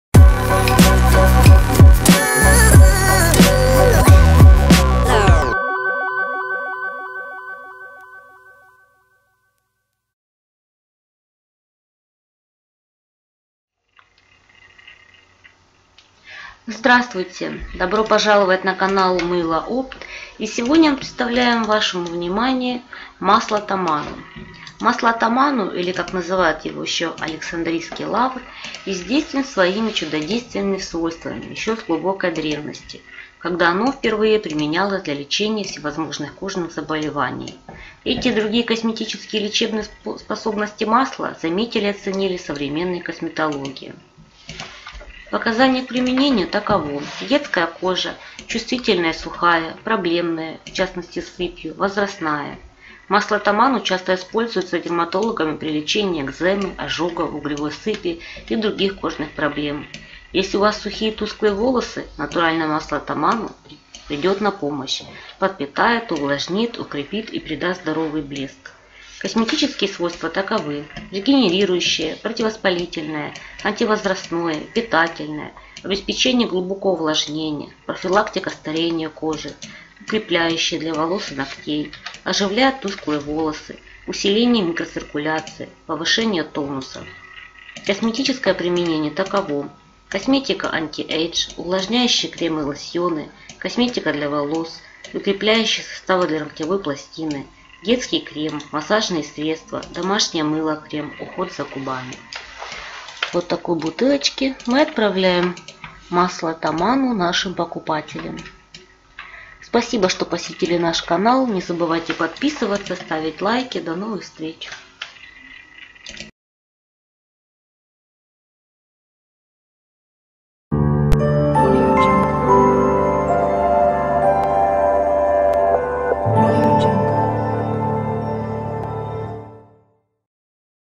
Средство от прыщей - Масло Таману
Видео обзор Средство от прыщей - Масло Таману от интернет магазина Мыло Опт